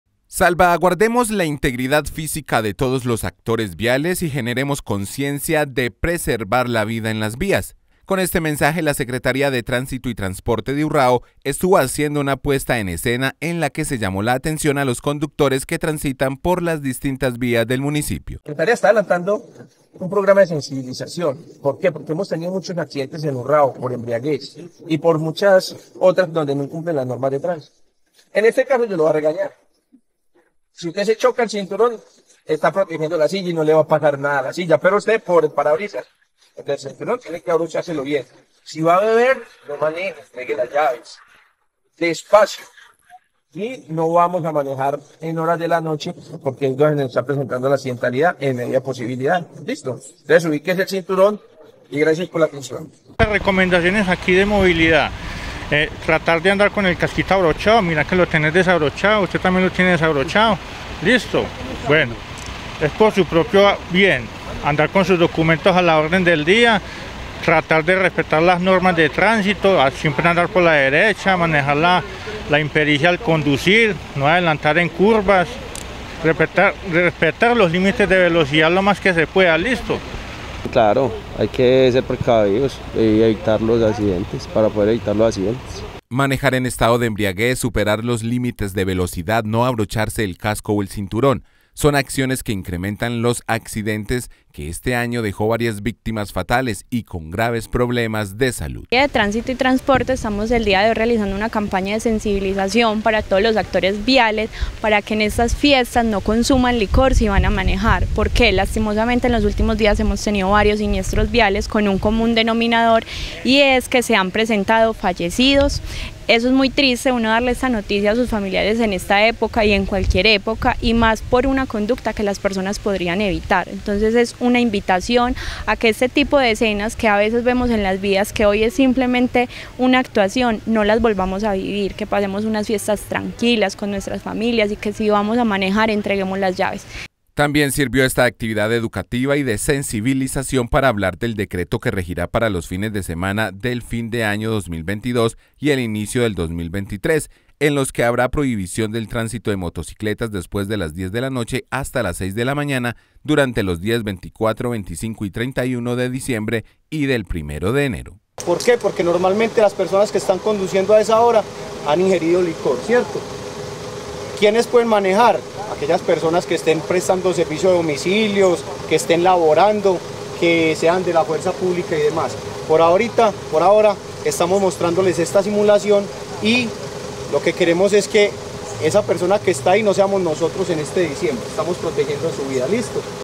Conciencia para preservar la vida en las vías
Las celebraciones de fin de año traen consigo consecuencias nefastas en materia de seguridad vial, accidentes de tránsito que acaban con vidas y la alegría de muchas familias, es por ello que se realizó una jornada de sensibilización en la que se invita a los actores viales a proteger su vida y las de todos en las carreteras.